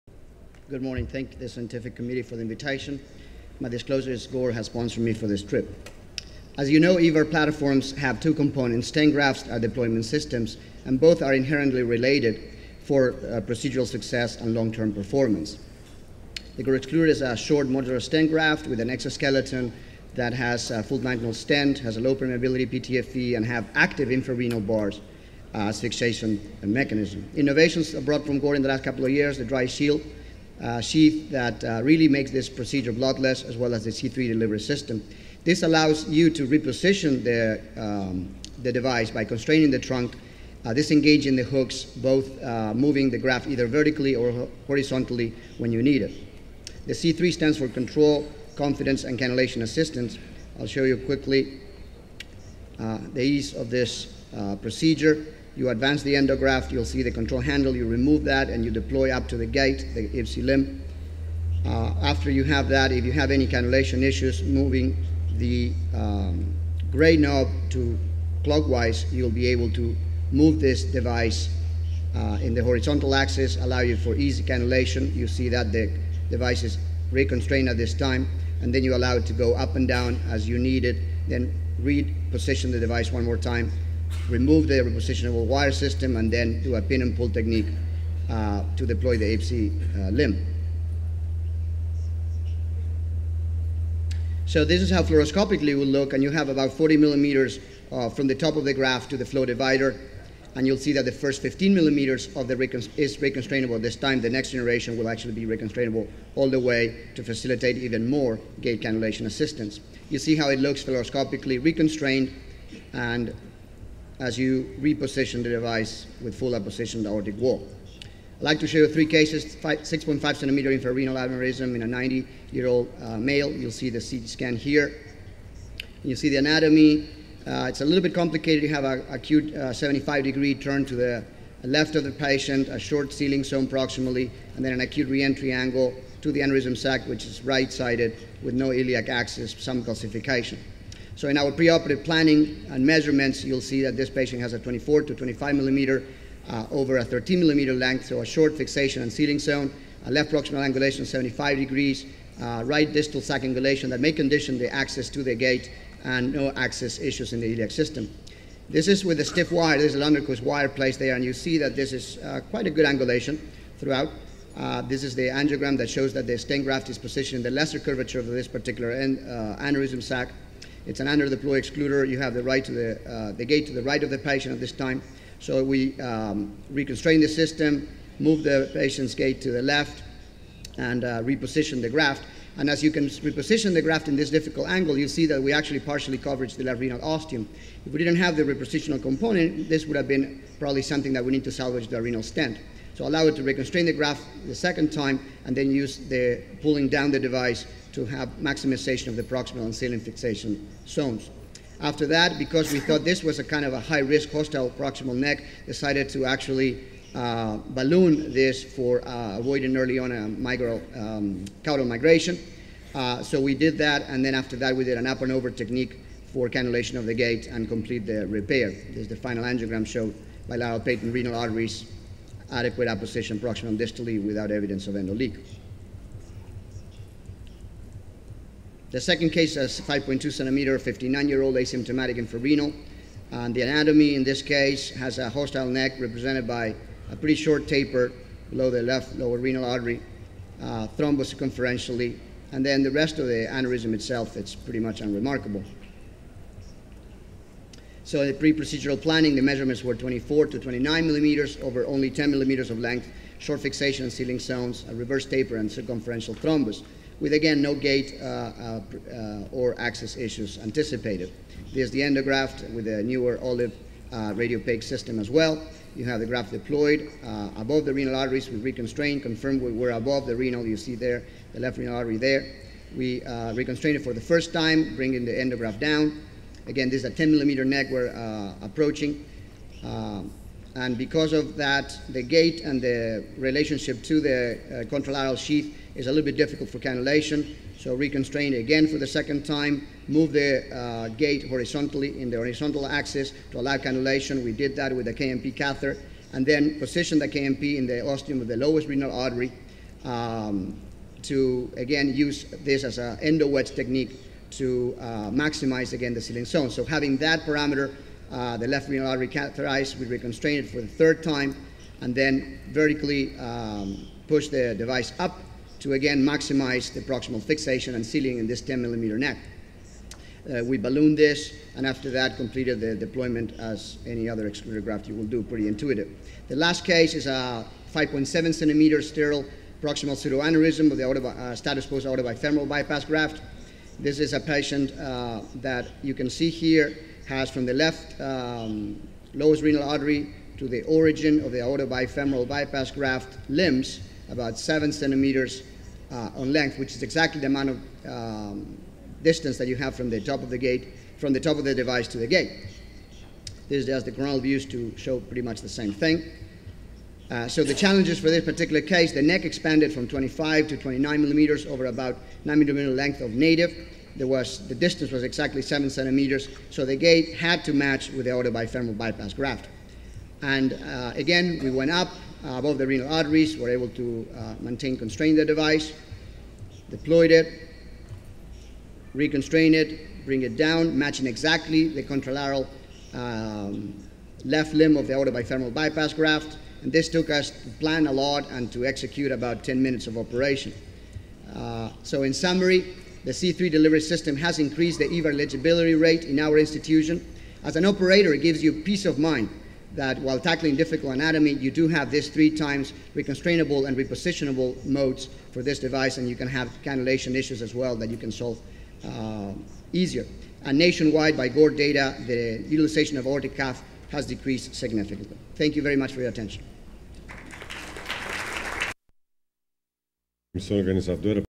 A endoprótese Gore C3 reposicionável
Encontro Carioca de Cirurgia Vascular 2012
Sessão II: Aorta
Tema: A endoprótese Gore C3 reposicionável
Palestrante: Christian Bianchi